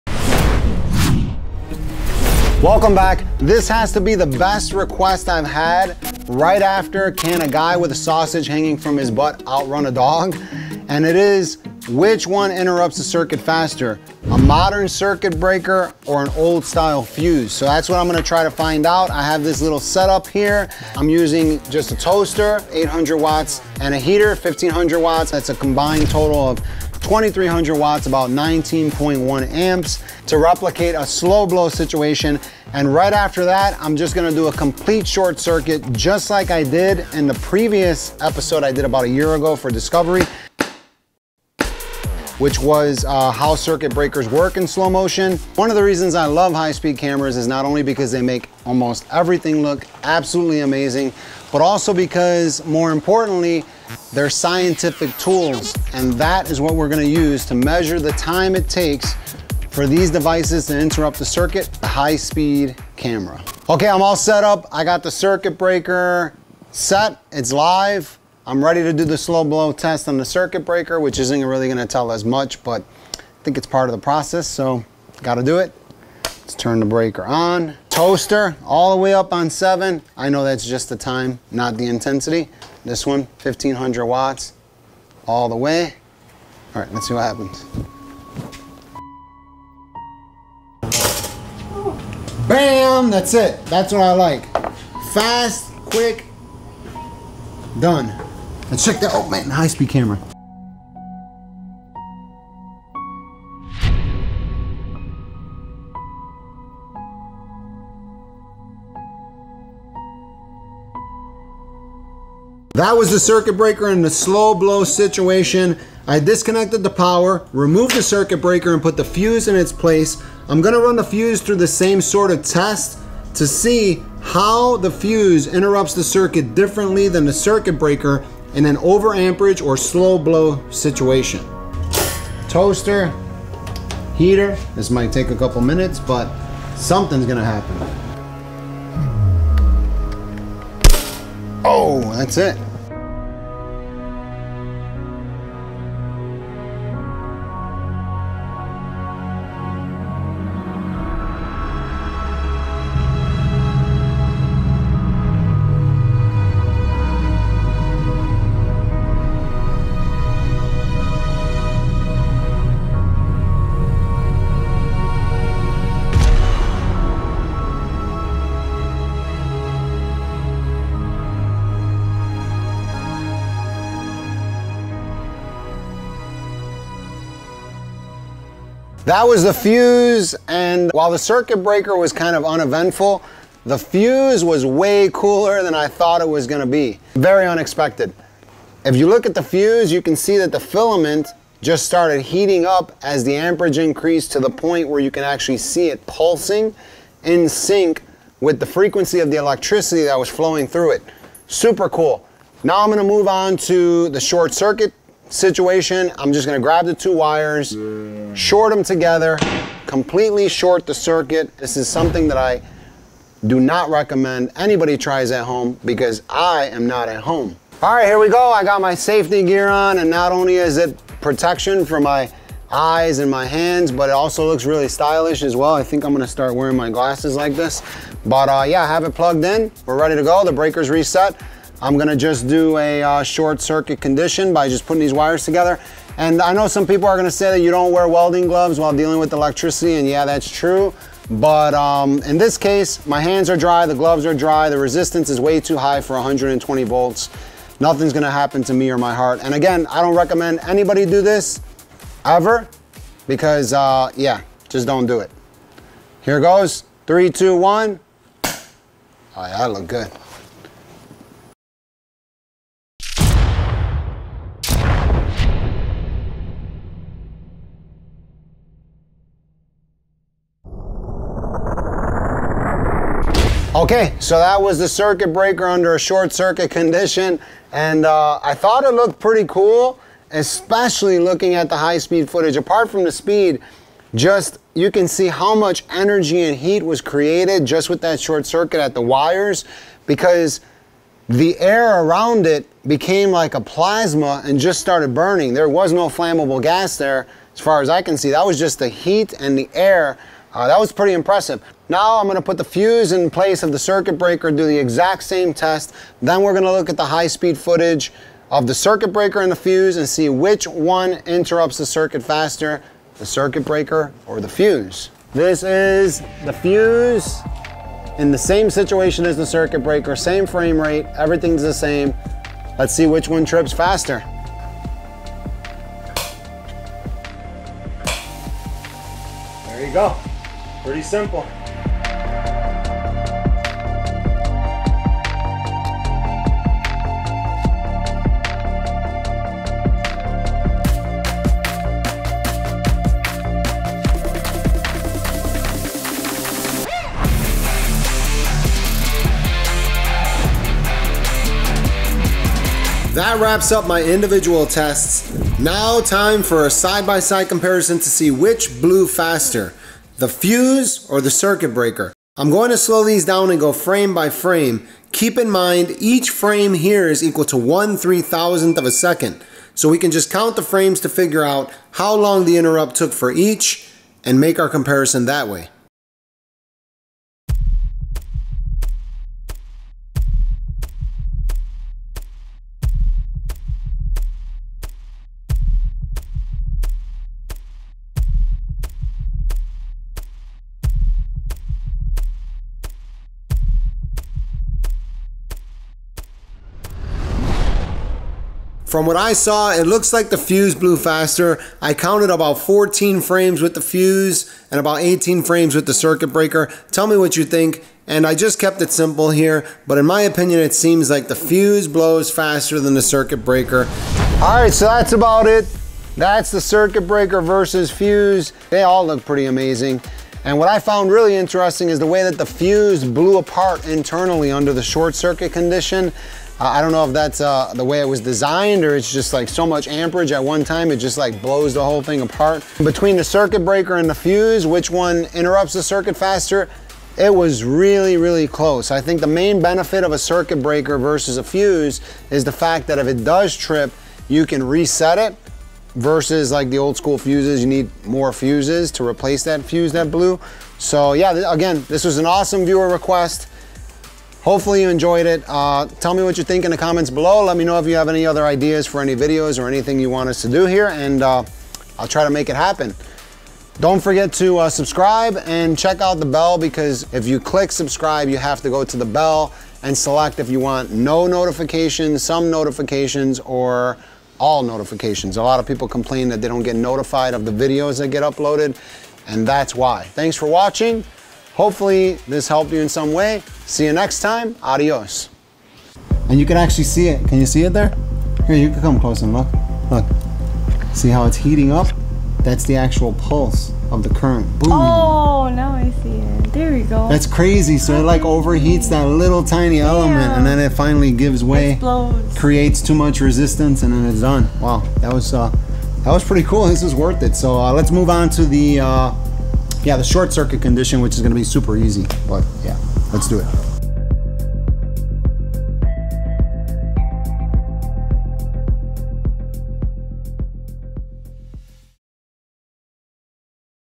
Fuse vs Circuit Breaker Which Blows Faster (Slow Motion)
In this episode I put a Screw-In Fuse up against a Modern Circuit Breaker and Film it with the High Speed Camera (super slow motion) to see which one trips faster in the event of a short circuit, a fuse or circuit breaker.

In slow motion you can also see how a circuit breaker or fuse works inside when it blows or trips.
I hope this video gave you a deeper and better understanding of how circuit breakers and fuses work inside as well as answer the viewers questions which one blows faster.

if you want to see how a circuit breaker works in general, here is the previous episode on how circuit breakers work in slow motion.

Watch this in 4k when possible, it looks incredibly awesome !

Produced By: Carbon 12

Directed By: Matt Mikka